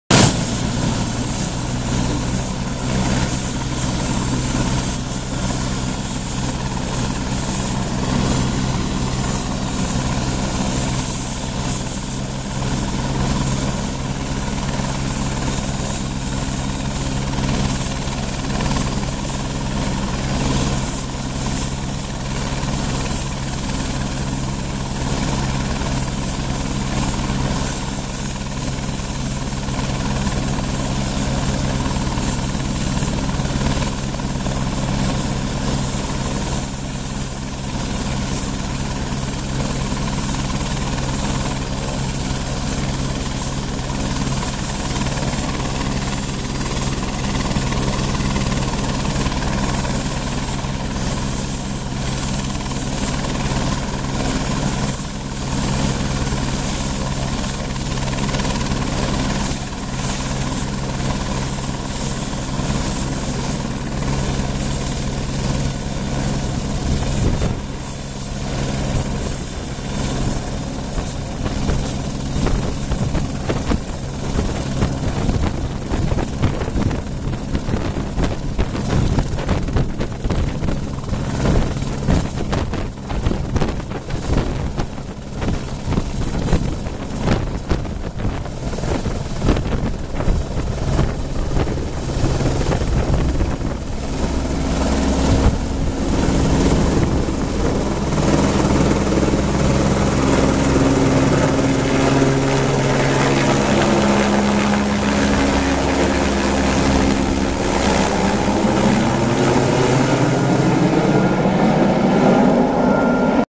PDG's G-NETR Departing Nevis Range
G-NETR one of PDG Helicopters AS-355's is seen here lifting from the Nevis Range car park and departing the area returning to EGPG Cumbernauld.